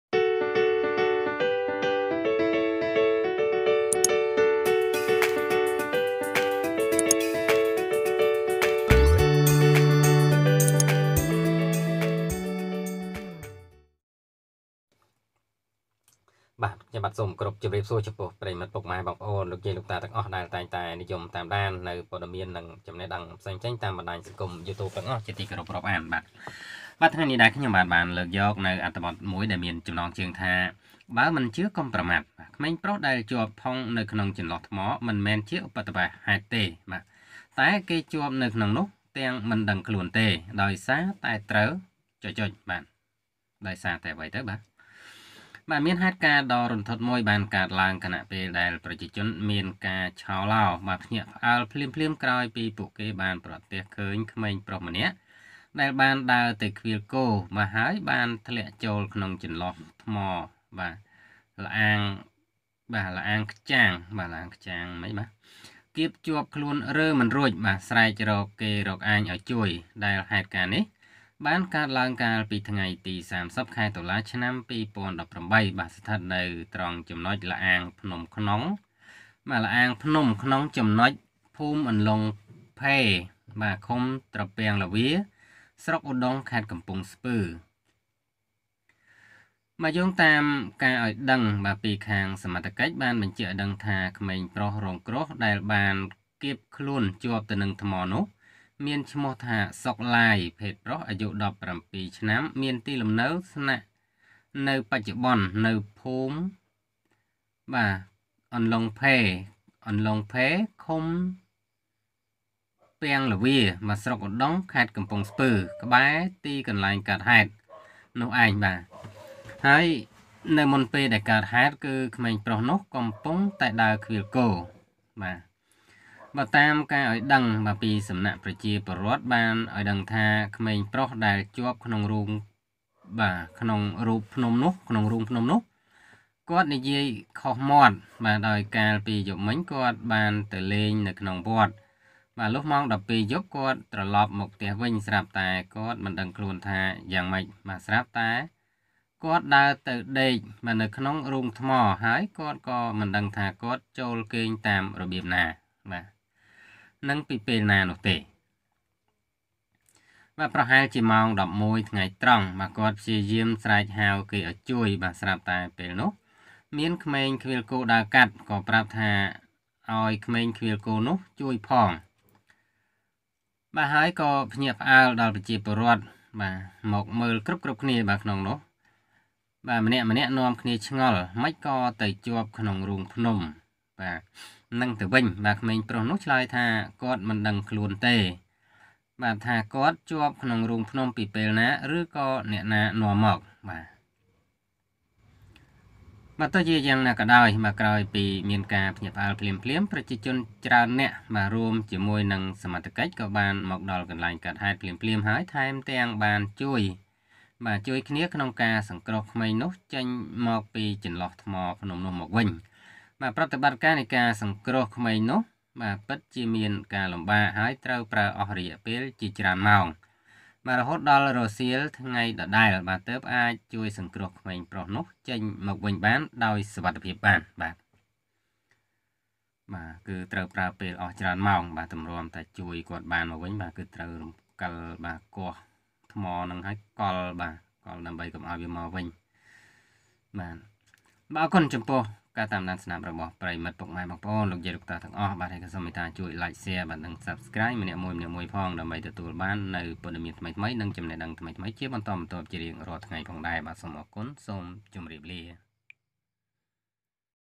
មិនជឿកុំប្រមាថ! ក្មេងប្រុសដែលជាប់ផុងនៅក្នុងចន្លោះថ្មមិនមែនជាឧបទ្ទេវហេតុទេ តែគេជាប់នៅក្នុងនោះទាំងមិនដ
មិនជឿកុំប្រមាថ! ក្មេងប្រុសដែលជាប់ផុងនៅក្នុងចន្លោះថ្មមិនមែនជាឧបទ្ទេវហេតុទេ តែគេជាប់នៅក្នុងនោះទាំងមិនដឹងខ្លួនទេដោយសារតែត្រូវ…….